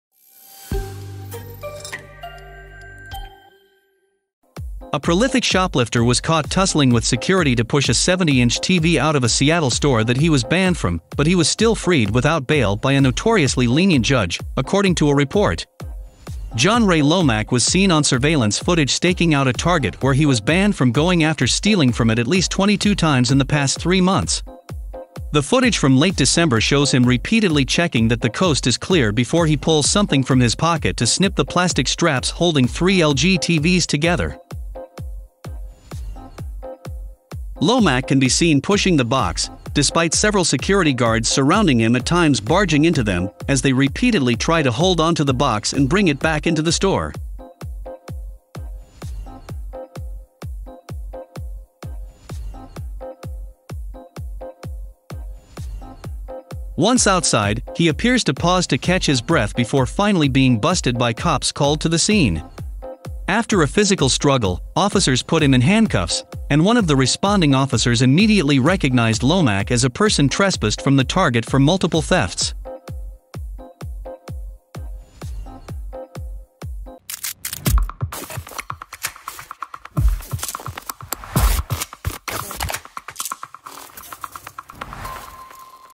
Man steals a 70-inch TV from the shop he had previously stolen at least 22 times in three months
A prolific shoplifter was caught pushing a 70-inch TV out of a Seattle store that he was banned from.